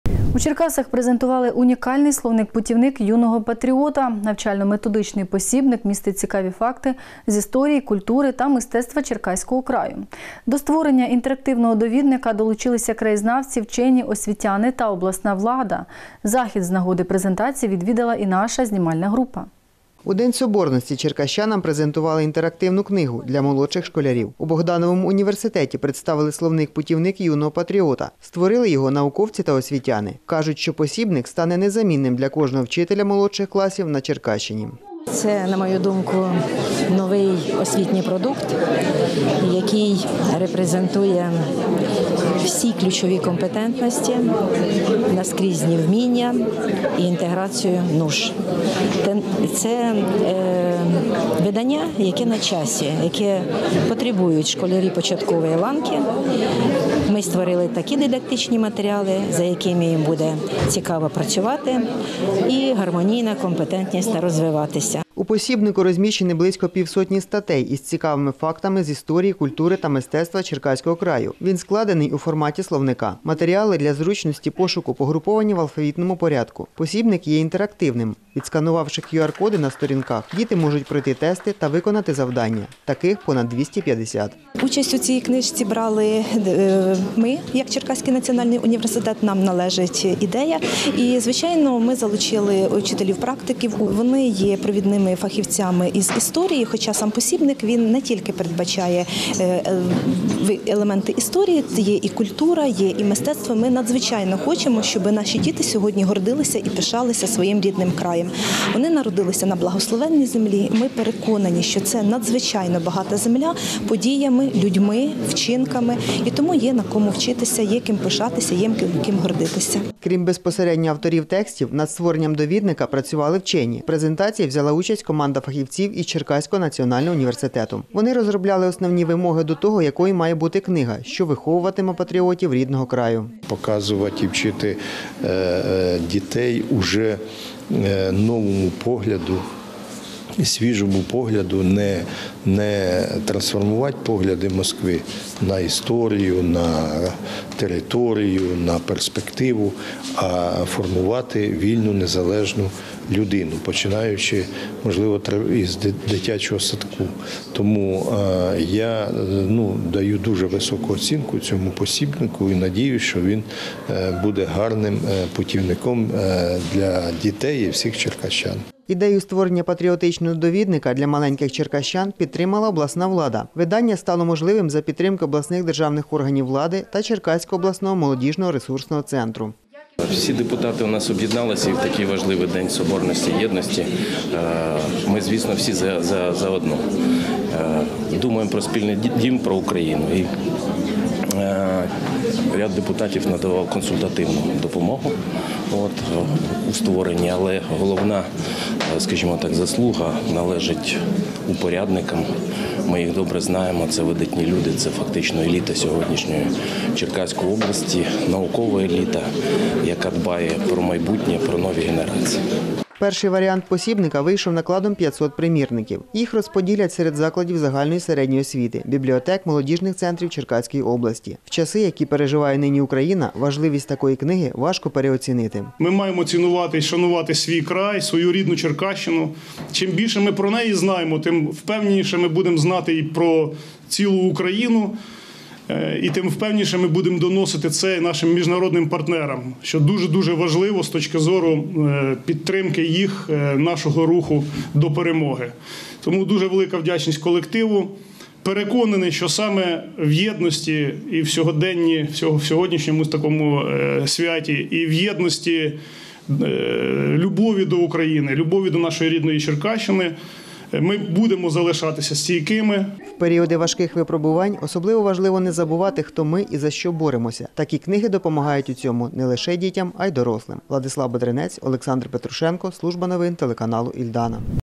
У Черкасах презентували унікальний словник-путівник юного патріота
Навчально-методичний посібник містить цікаві факти з історії, культури та мистецтва Черкаського краю. До створення інтерактивного довІдника долучилися краєзнавці, вчені, освітяни та обласна влада.

Захід з нагоди презентації відвідала і наша знімальна група.